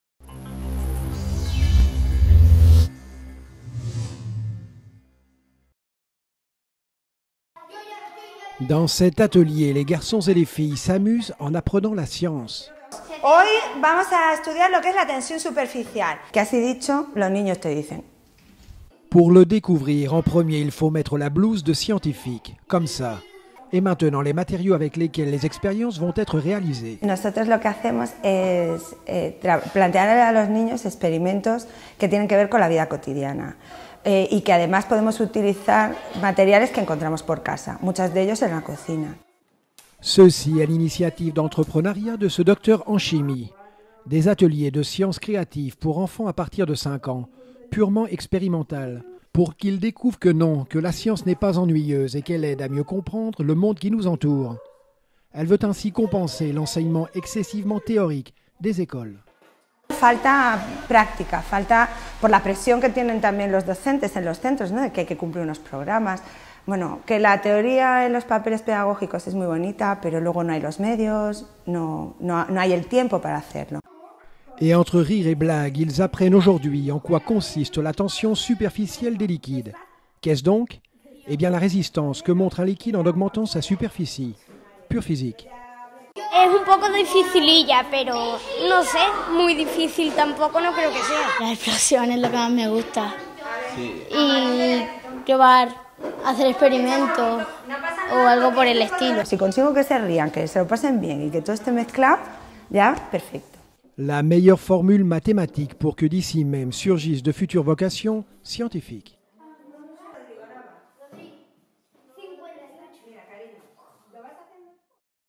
NA Des ateliers de science quotidienne pour éveiller des vocations chez les futures génération
Petits Alchimistes est le projet d’Almudena Gil, un docteur en Sciences Chimiques installée à Séville, fatiguée d’entendre dire que la science est ennuyeuse. Almudena réalise des ateliers pour garçons et filles à partir de cinq ans où elle introduit, au travers d’expériences simples et amusantes, les petits alchimistes aux principes de la Physique et de la Chimie.